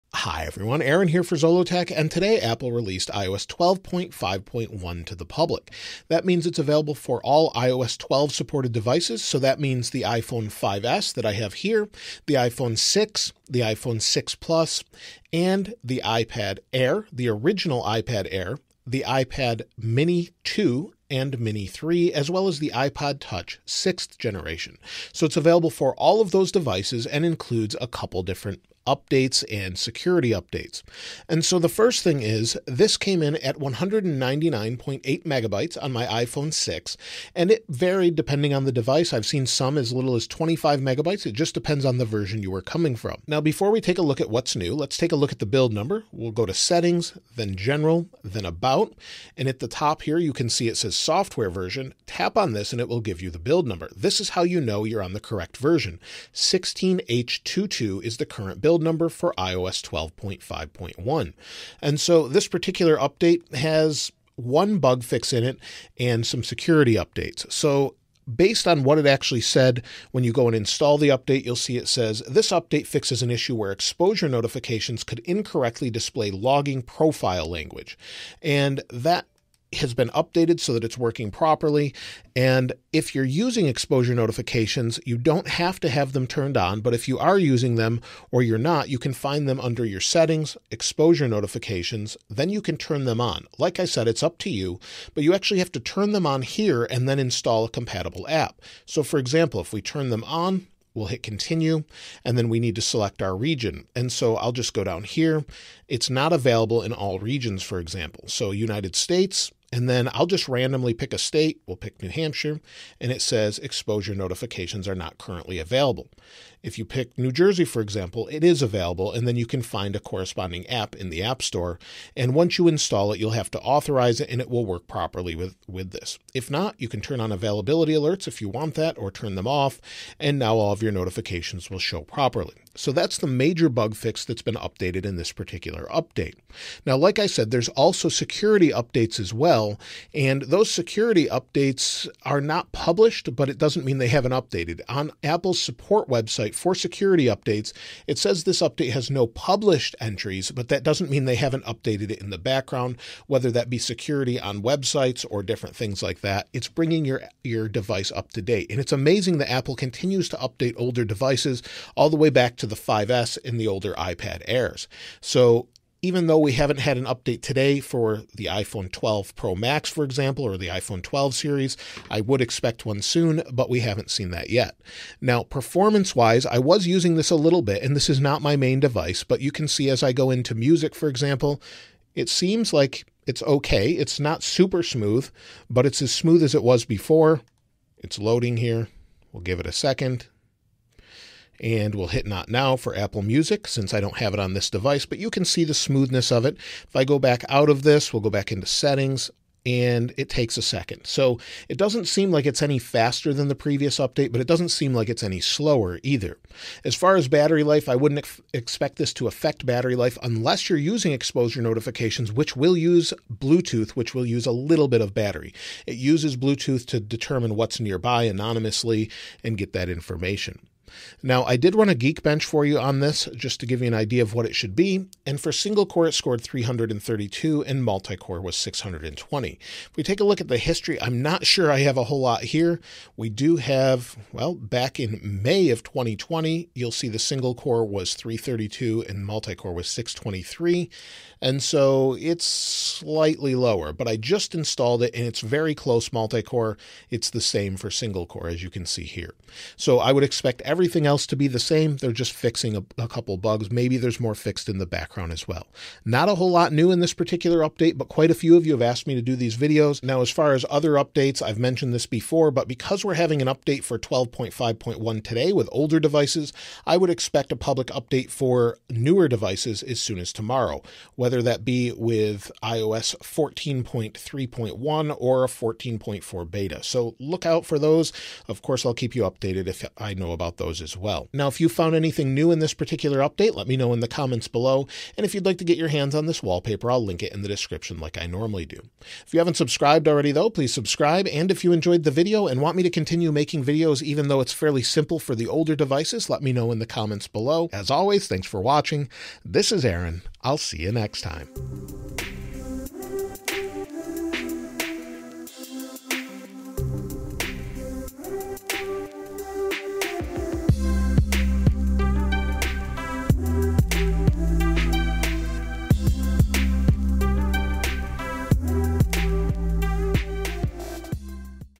iOS 12.5.1 is Out! - What's New?
iOS 12.5.1 released for all supported devices and brings with it a bug fix and security updates for iPhone and iPad. In this video I go over all the features, updates and changes in iOS 12.5.1 and talk about when to expect iOS 14.3.1 or iOS 14.4 using, iPhone 12 Pro Max, iPad Air 2, iPhone 5s and iPhone 6

***Time Codes***
00:00 - Introduction
00:09 - Supported devices
00:31 - Size
00:43 - Build number
01:04 - Bug fixes
02:16 - Security updates
02:54 - Performance
03:26 - Battery life
03:40 - Benchmarks
04:20 - iOS 14 updates
04:41 - Conclusion
05:01 - Outro
05:21 - End